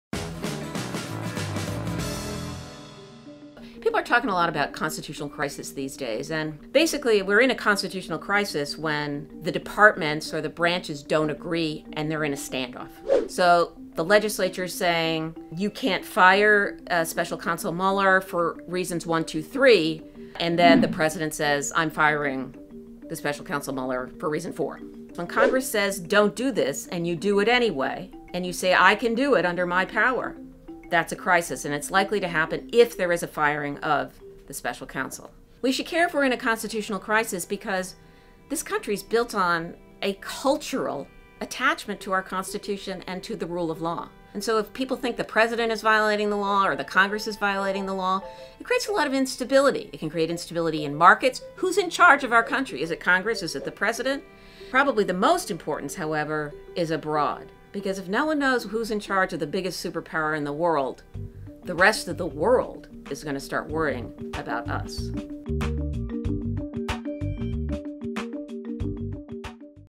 Legalese: Victoria Nourse on 'Constitutional Crisis'
If President Trump moves to fire Special Counsel Robert Mueller, it could trigger a constitutional crisis. What does that mean? Professor Victoria Nourse explains in our latest